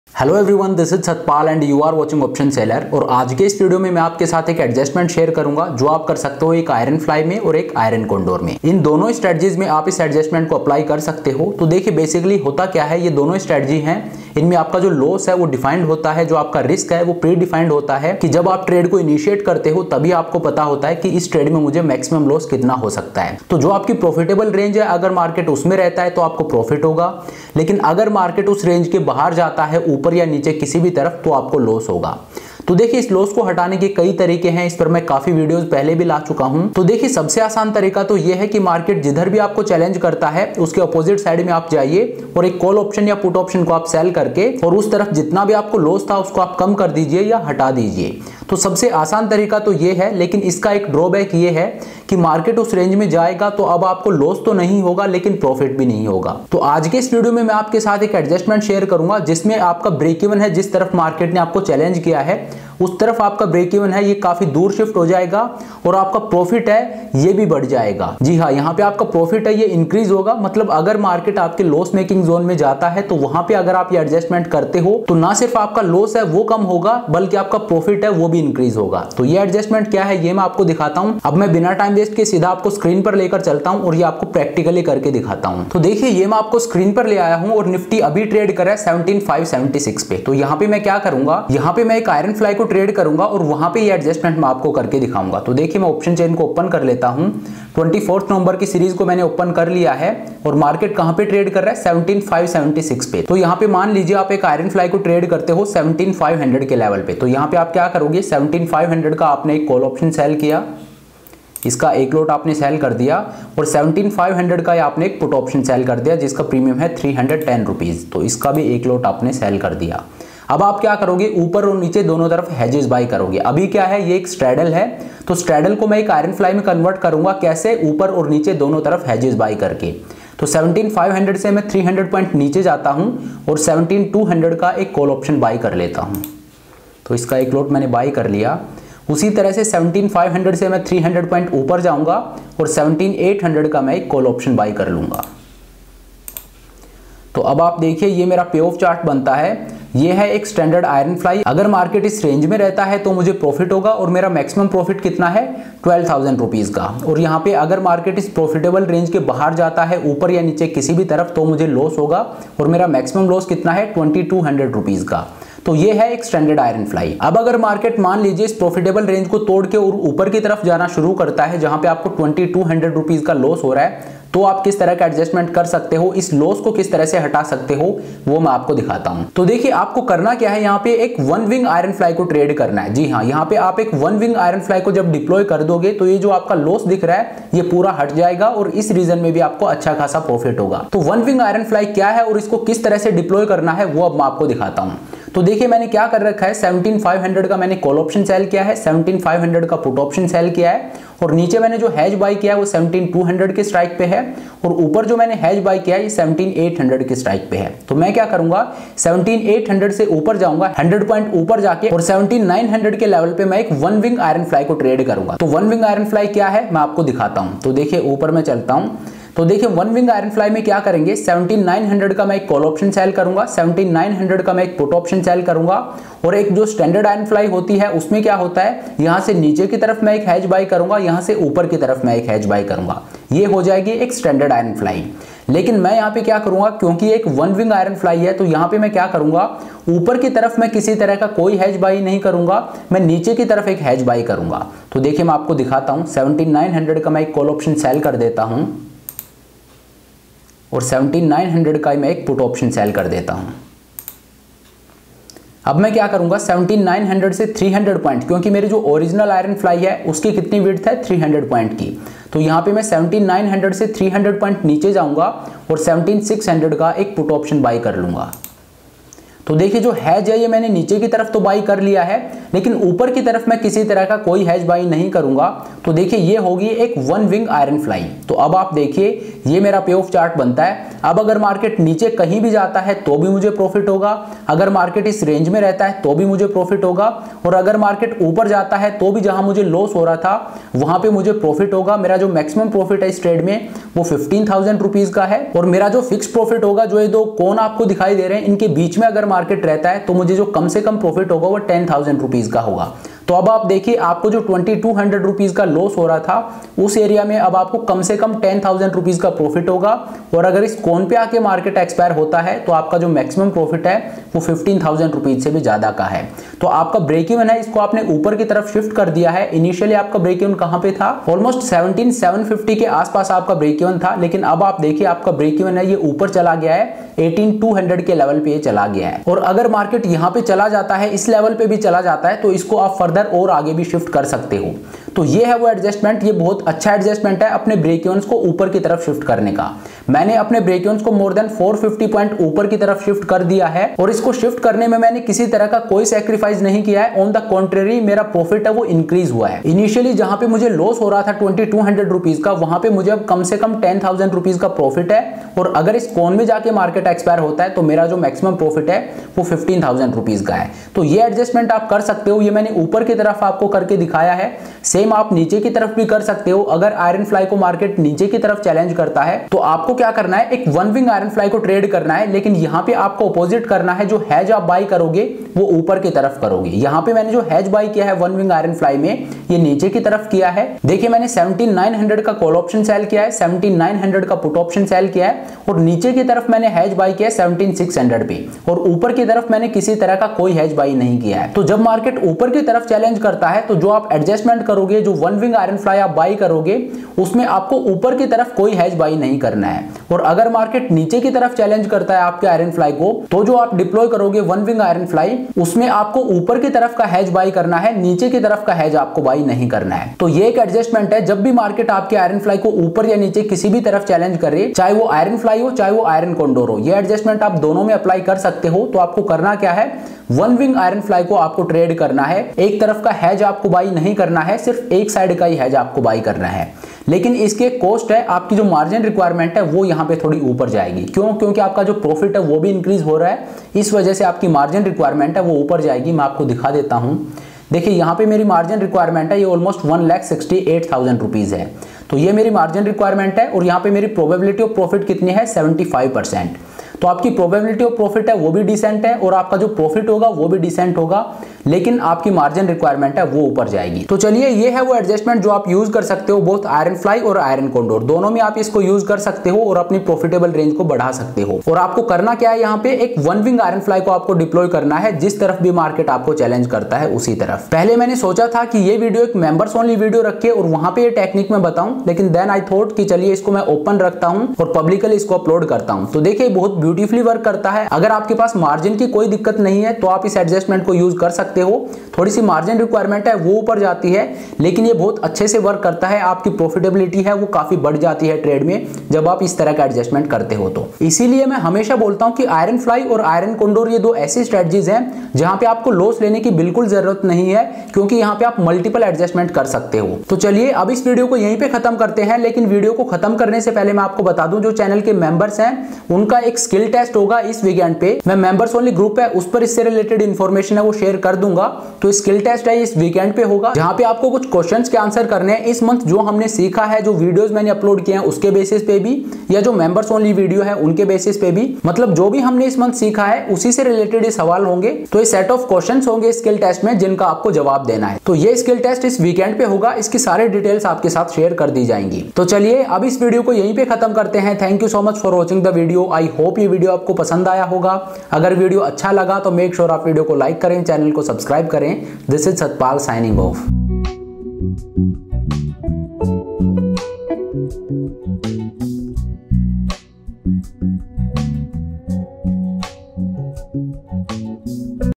Best Adjustment For IRONFLY & IRON CONDOR | Mastering Options Adjustments | Option Sailor
In this video I will share a very unique way of doing adjustments in Ironfly & Iron condor.

Option Sailor